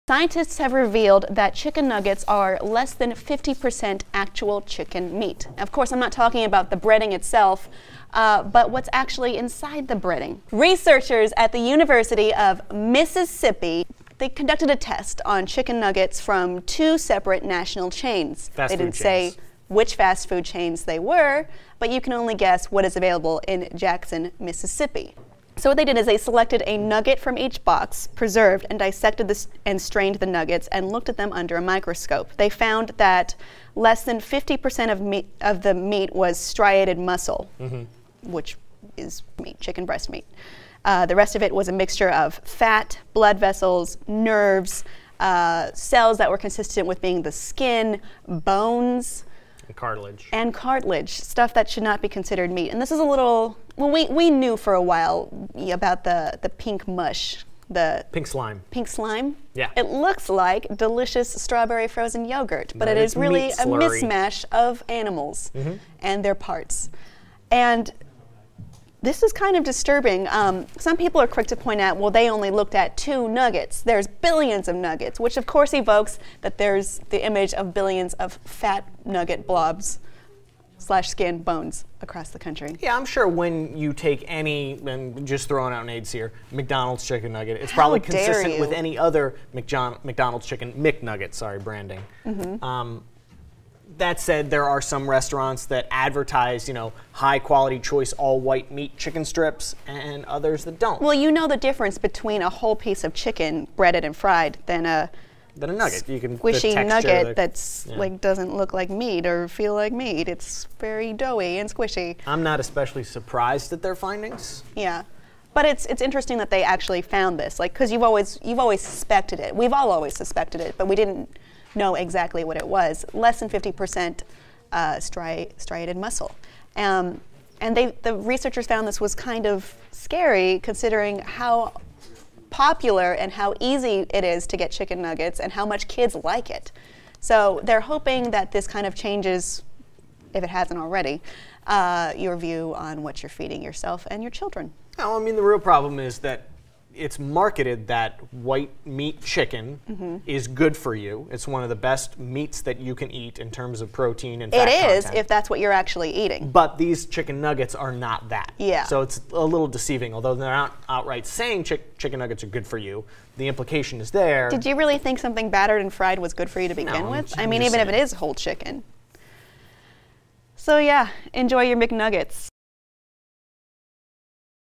Scientists Find Out What Chicken Nuggets are REALLY Made Of
We all know that chicken nuggets don't contain the best meat. Or should we say, "meat" with sarcastic quotes? Researchers from the University of Mississippi in Jackson broke down some chicken nuggets to see what they were really made of, specifically. What they found was that about 60 percent of a tested nugget was NOT meat. Then...what were the other ingredients? Kim Horcher and Tim Frisch discuss.